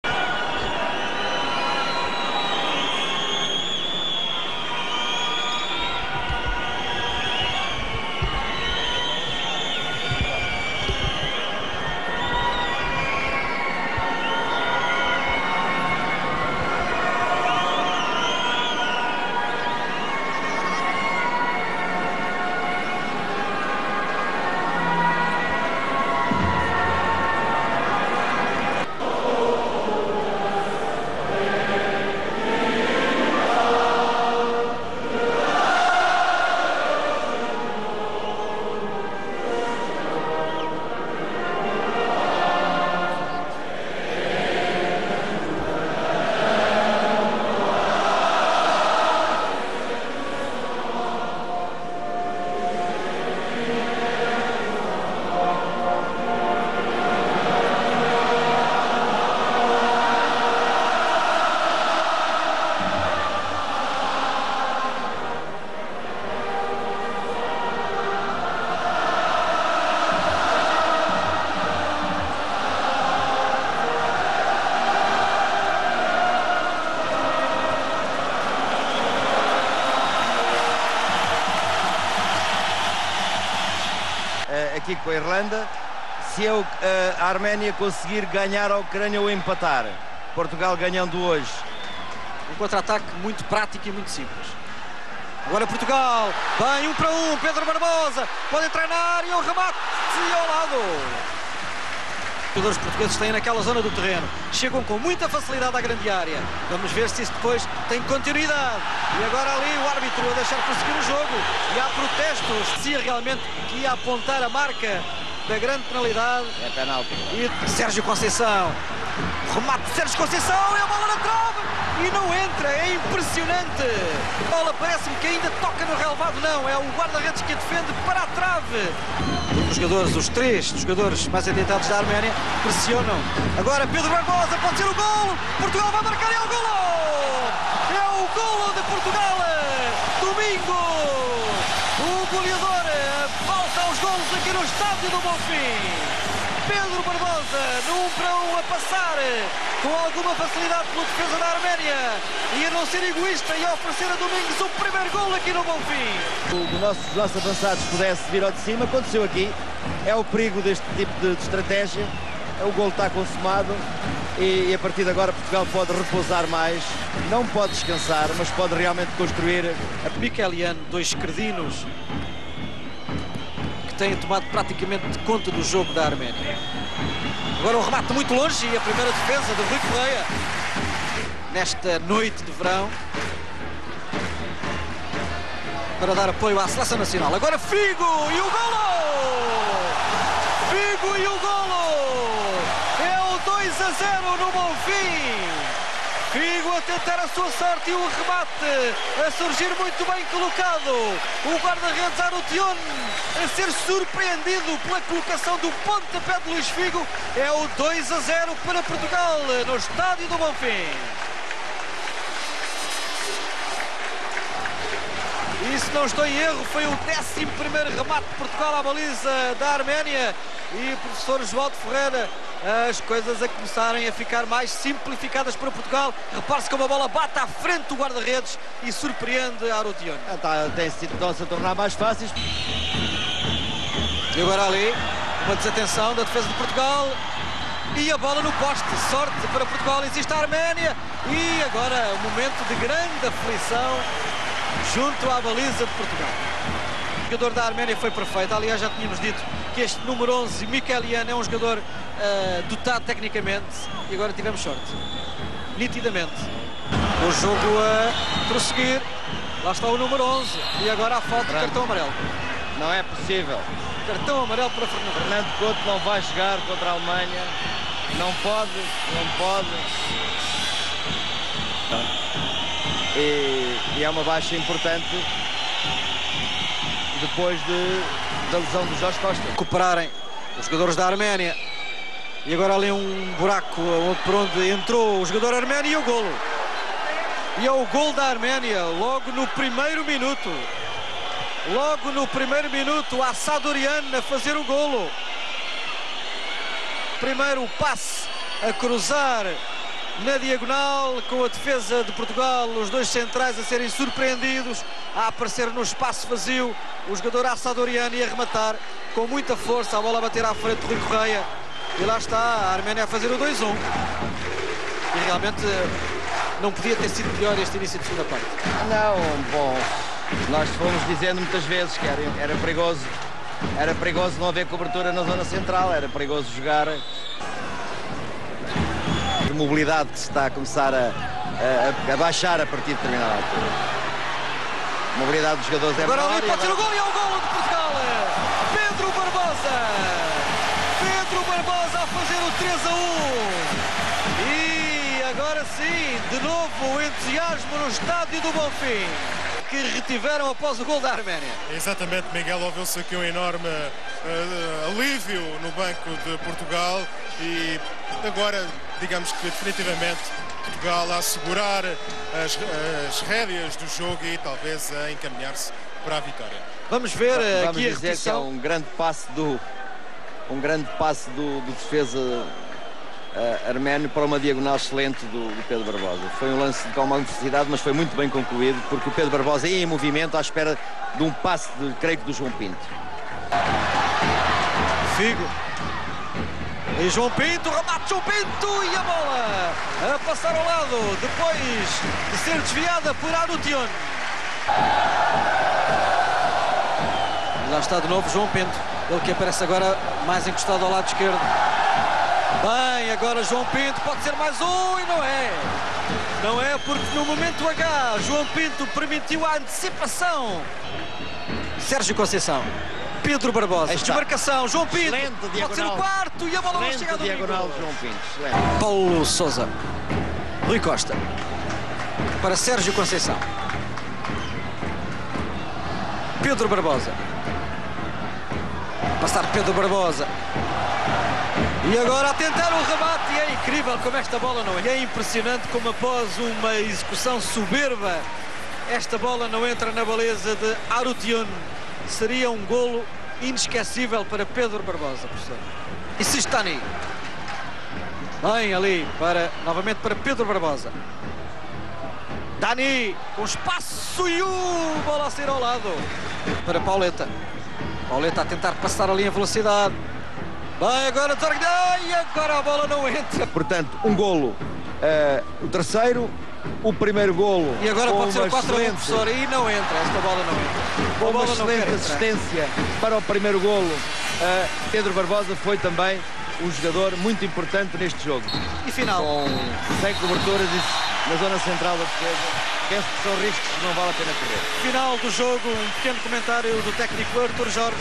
No 28. PORTUGAL vs ARMENIA 3-1 (20/08/1997) Highlights
20/08/1997, Setubal, "Estadio do Bonfim", 16.000 attendance
WORLD CUP 1998 QUALIFIERS

PORTUGAL 3:1 ARMENIA

PORTUGAL
1. Rui Correira
2. Sergio Conceiçao
3. Paulinho Santos (Dimas '83)
4. Hélder
5. Fernando Couto
6. Paulo Sousa
7. Luis Figo
8. Joao Pinto (Dani '67)
9. Domingos (Pauleta '78)
10. Rui Costa
11. Pedro Barbosa
Head Coach: Artut Jorge

ARMENIA
1. Harutyun Abrahamyan
3. Rafael Nazaryan
2. Felix Khodzhoyan
4. Sargis Hovsepyan
5. Hakob Ter-Petrosyan (Artur Petrosyan '46)
6. Harutyun Vardanyan
7. Garnik Avalyan (Hayk Gevorgyan '80)
8. Albert Sarkisyan (Tigran Gspeyan '83)
9. Tigran Yesayan
10. Eric Assadouryan
11. Karapet Mikaelyan
Head Coach: Khoren Hovhannisyan

GOALS:
1:0 Domingos '23
2:0 Luis Figo '31
2:1 Eric Assadourian '46
3:1 Pedro Barbosa '55